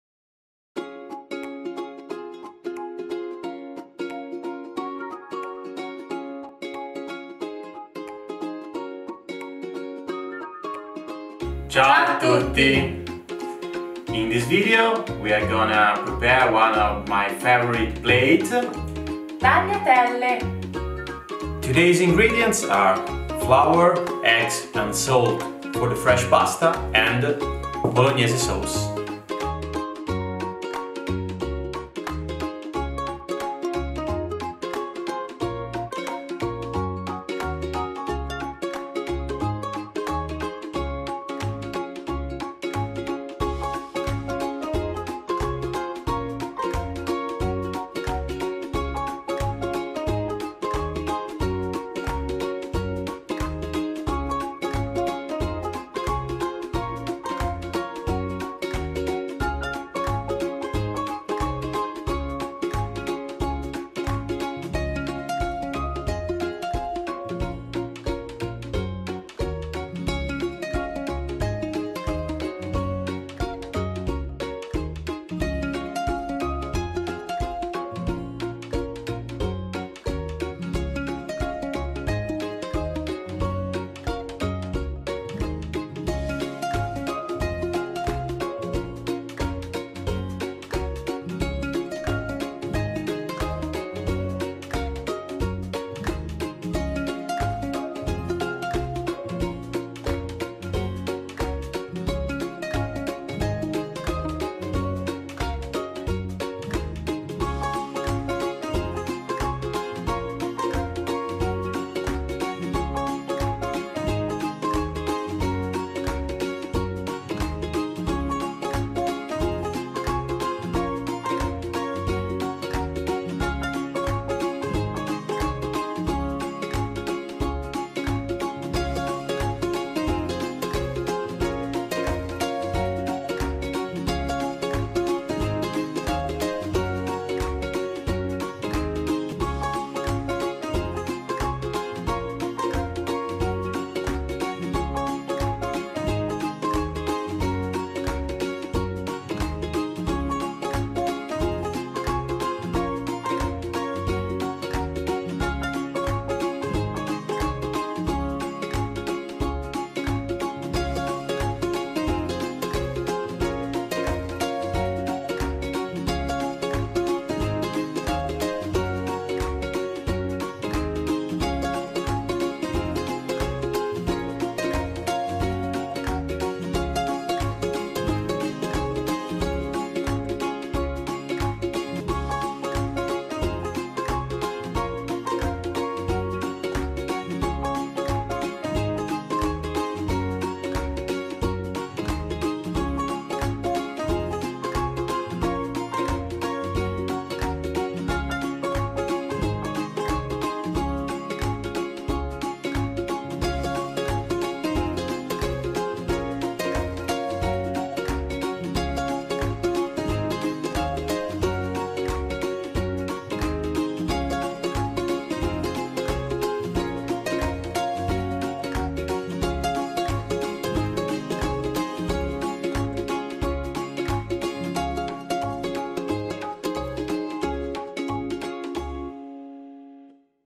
Easy Tagliatelle Recipe in English - Fast and Tasty
Our Grandma's recipe for Tagliatelle with bolognese sauce. Fresh egg Tagliatelle pasta